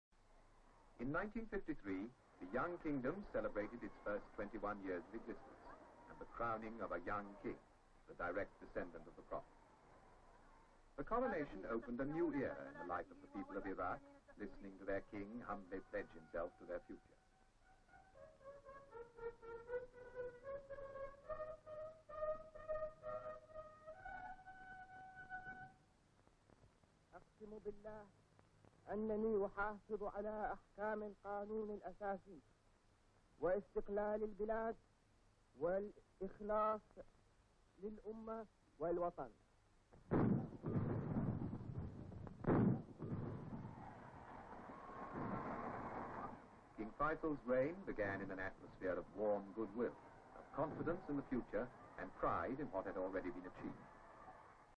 Coronation of King Faisal II in Iraq. Archive film 93032
Iraq.  A parade in the streets at 1953 celebrating 21 years of independence and the crowning of King Faisal II - showing him in Royal horse and carriage. Now there is a radio broadcast, a crowd. Close up of people listening.  Field gun shooting in a ceremony of goodwill as the king comes out from the palace.